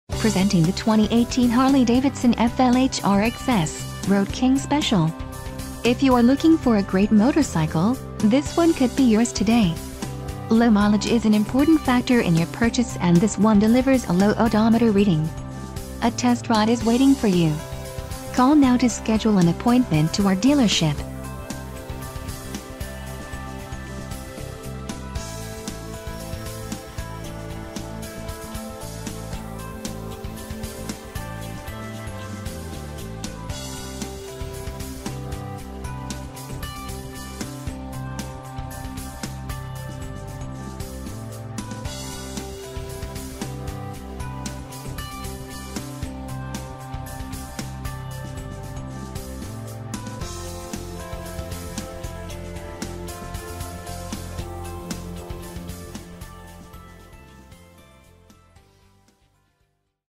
2018 Harley-Davidson FLHRXS - Road King Special - Motor ...
Join the Motor City Family Today!!2018 Harley-Davidson Road King SpecialIf you want to know what ruling the road truly feels like, reach for the mini-ape handlebars on the ROAD KING Special motorcycle and send some fuel to its Milwaukee-Eight 107 engine.Features may include:Mini-Ape Handlebars9-inch tall, 1.25-inch wide mini-ape handlebars so you can sit taller, feel more assertive, and ride more aggressively.Milwaukee-Eight V-Twin EngineA powerful, cool-running engine with crisp throttle response and a pure, soul-satisfying rumble.Blacked-Out Custom StylingThis machine is end-to-end blacked-out, stripped-down bare muscle from the aggressive turbine wheels to the tapered mufflers.Stretched SaddlebagsA sleek look on the outside and roomy on the inside with more than enough space to stash your riding essentials.